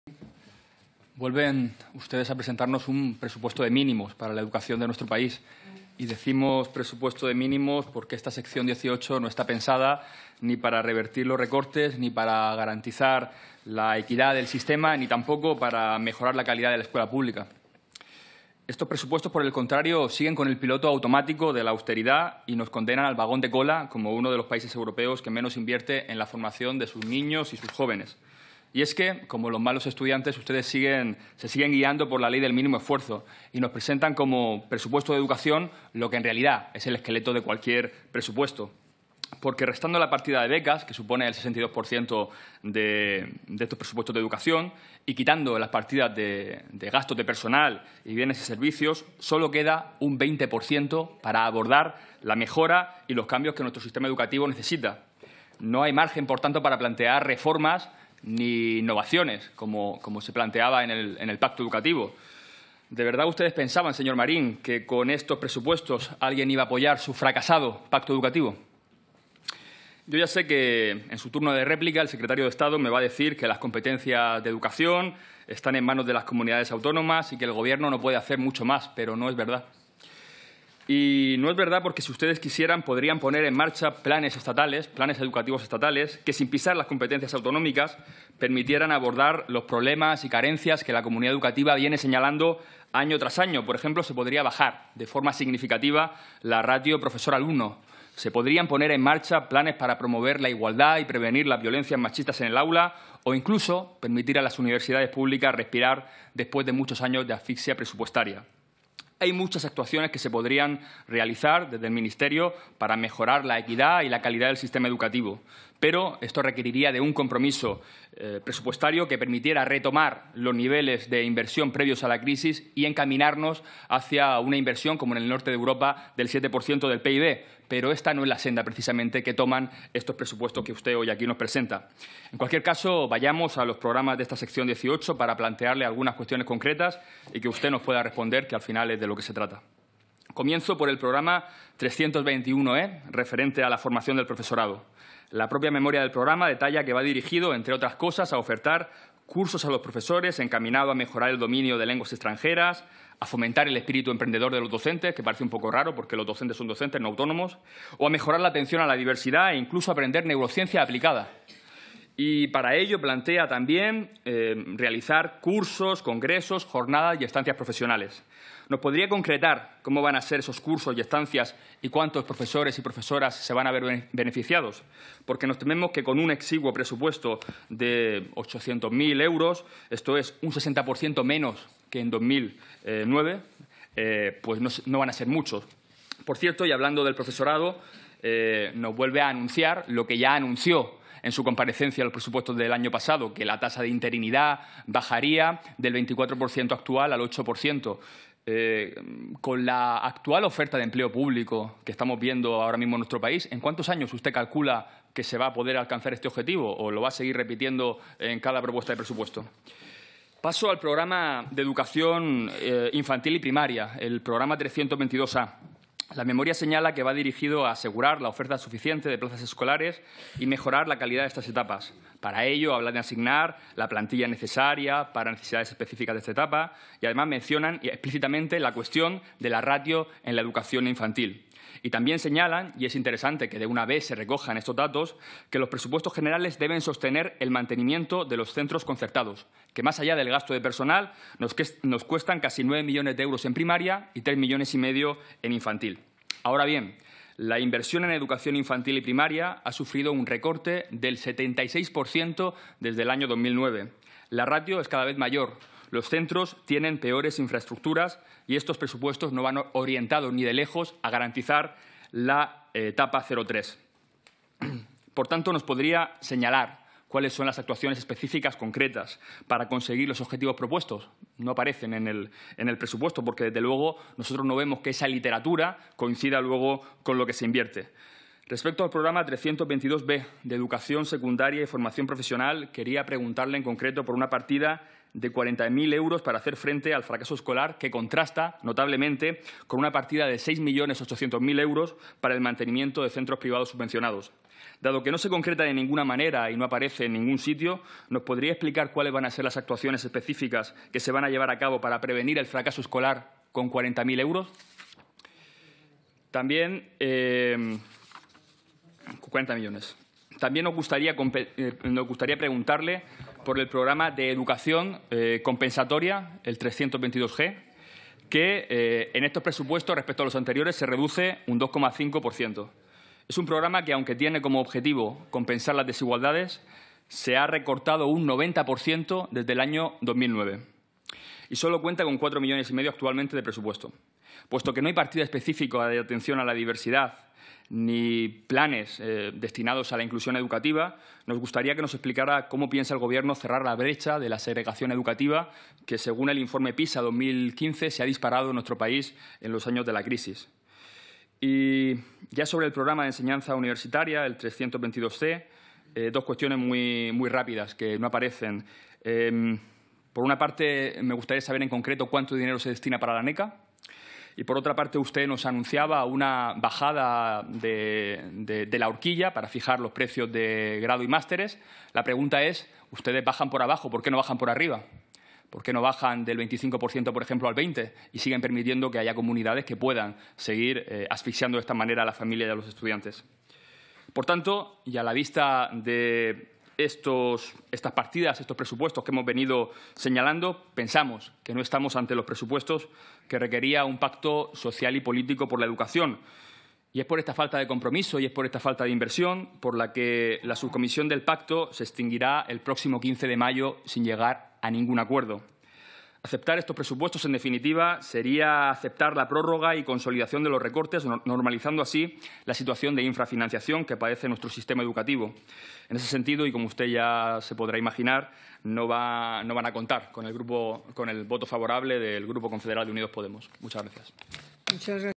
Javier Sánchez en la Comision de Educación y Deporte el 18 de abril
Durante la comparecencia del Secretario de Estado de Educación, Formación Profesional y Universidades.